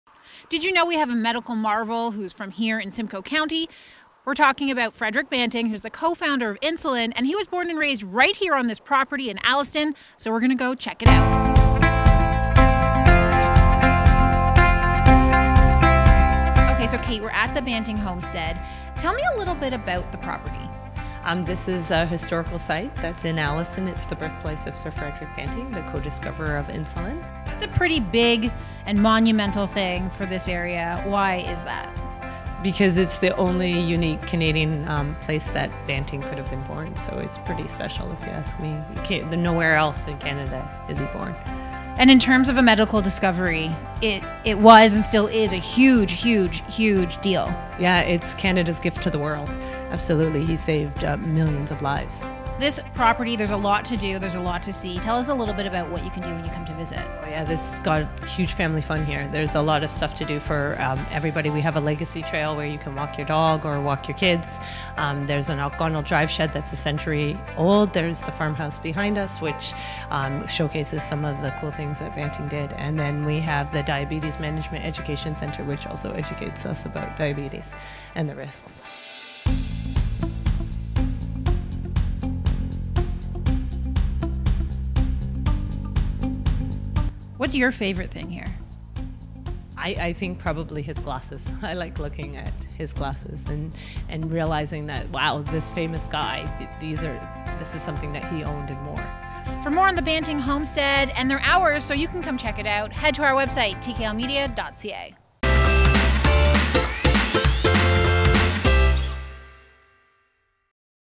Banting Square - Alliston's Condominium Community explores Banting Homestead
Banting Square is inspire the Sir Fredrick Banting. The team from This Kinda Life visit the Banting Homestead which is just around the corner for the Banting Square Community.